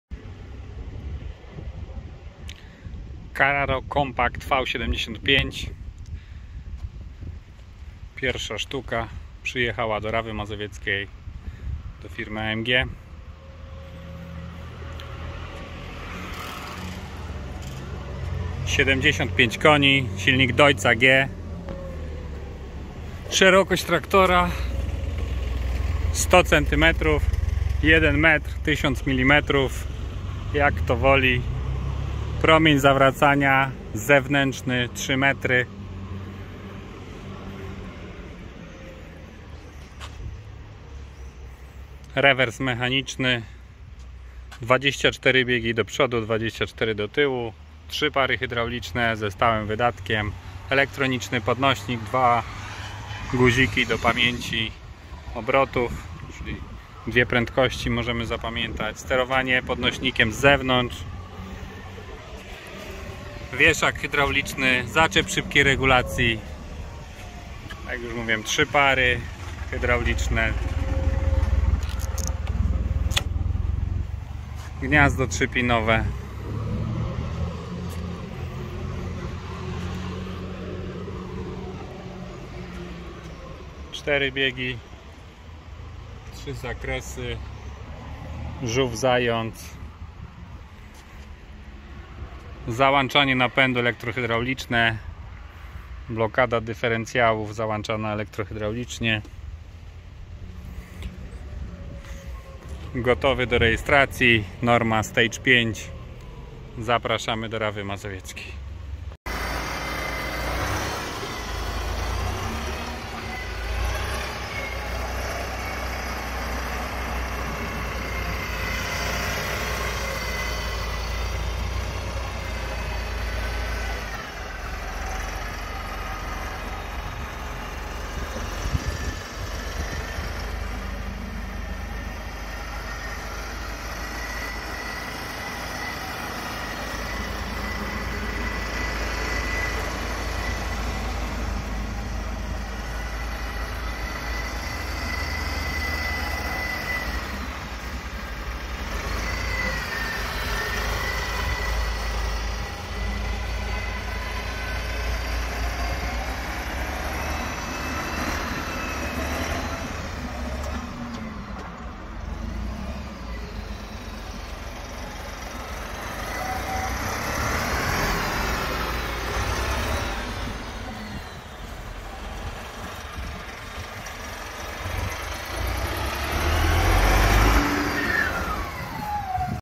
Prezentacja Carraro Compact V75 | 1m szerokości do Winnicy oraz Borówki
Prezentacja Carraro Compact V75 | 1m szerokości do Winnicy oraz Borówki , promień zawracania 3m !